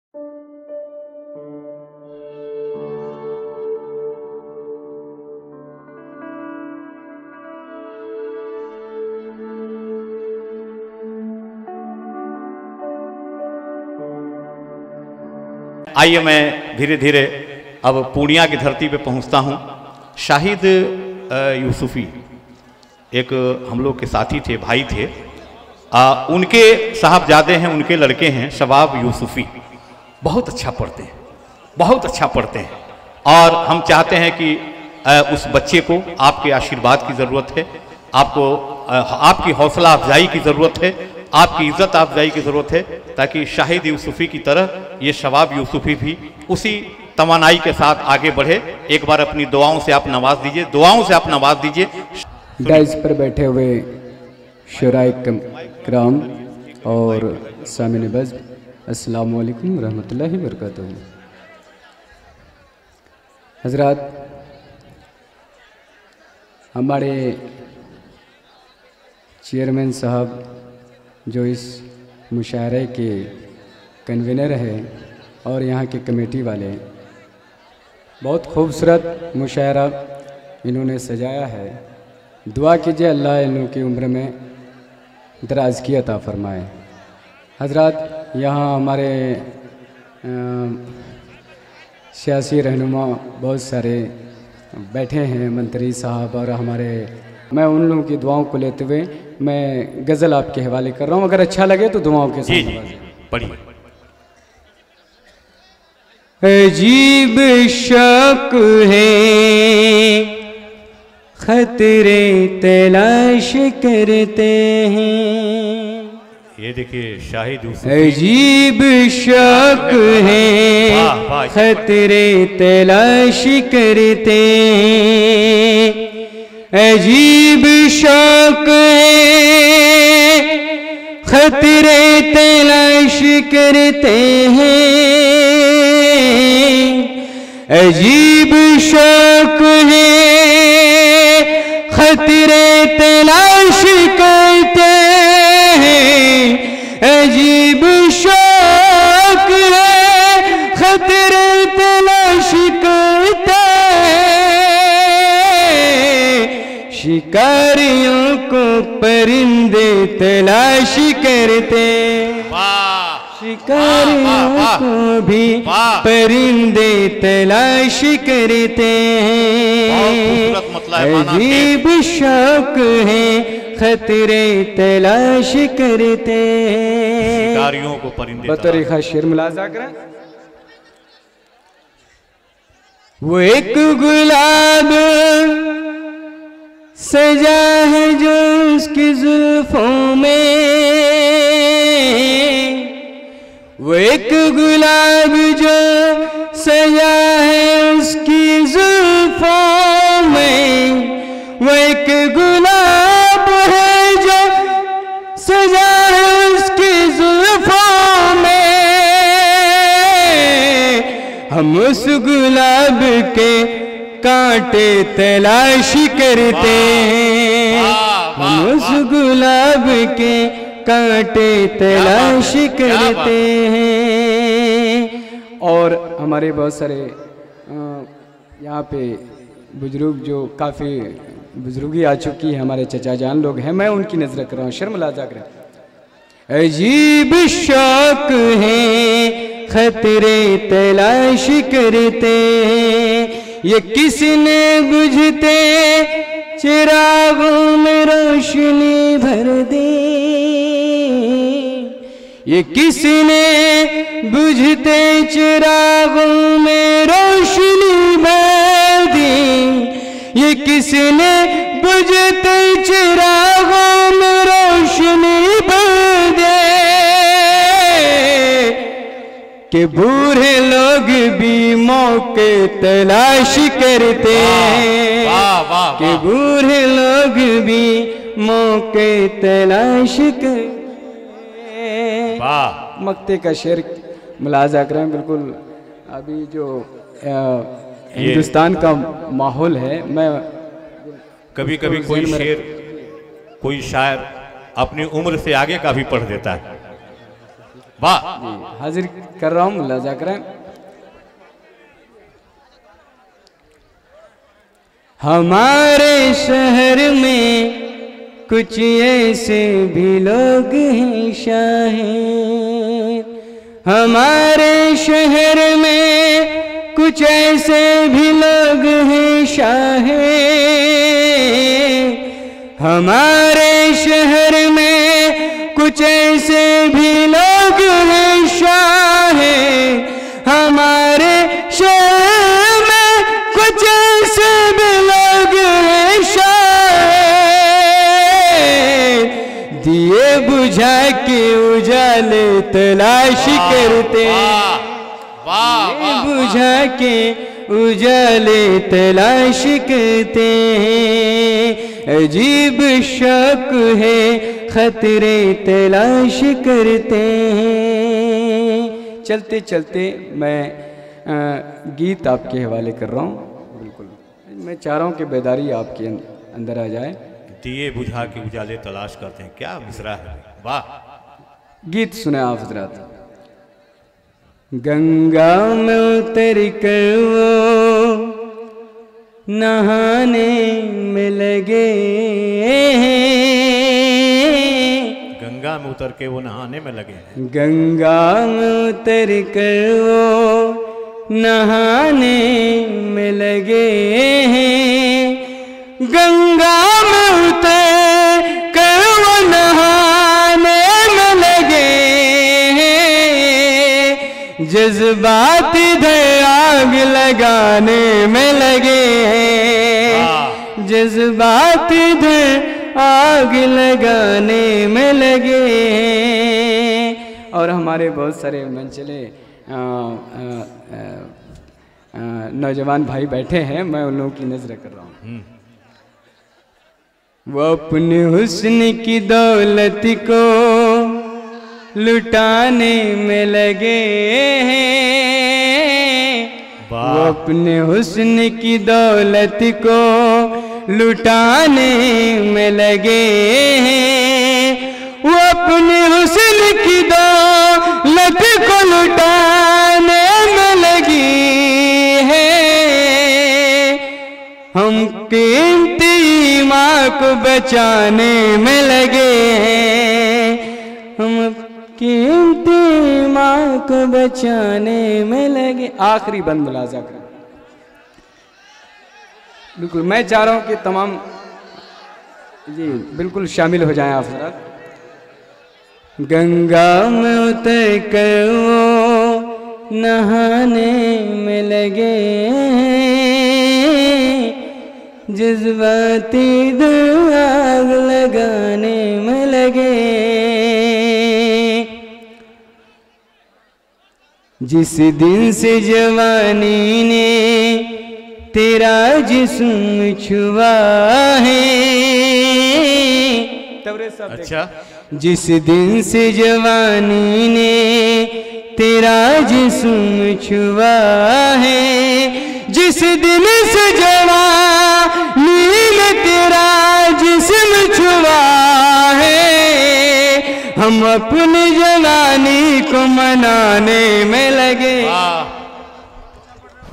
Shabab Yusufi, Bahadurganj Kishanganj Mushaira 2016, Mushaira Media
Shabab Yusufi
All India Mushaira, Nagar Panchayat Bahadurganj, Dist: Kishanganj
President Kul Hind Mushaira: MUJTABA ANWAR RAHI Sb, Nagar Panchayat Chairman Bahadurganj
General Secretary, Kul Hind Mushaira: EQRAMUL HAQUE BAGHI Sb, R.J.D. Leader
Sadar E Mushaira: Islamuddin Baghi Saahab, Ex-Kalyan Mantri
Convenor E Mushaira: Janaab Tabrez Hashmi Saahab
Chief Guest:
Dr. Abdul Gafoor Saahab, Alp Sankhyik Kalyan Mantri Bihar
Islamuddin Baghi Saahab, Ex-Kalyan Mantri Bihar
Guest Of Honor:
Vidhatak Naushad Aalam Thakurganj
Vidhayak Master Mujahid Aalam Kochadhaman
Akhtarul Iman Saahab, Ex-Vidhayak, Riyasati Sadar, Majlis Ittehadul Muslimeen, Bihar
Akhtarul Islam Shaheen, Vidhayak, Samastipur
Bhai Azeem
Iftekhar Ismail
Dr. Iftekhar Aalam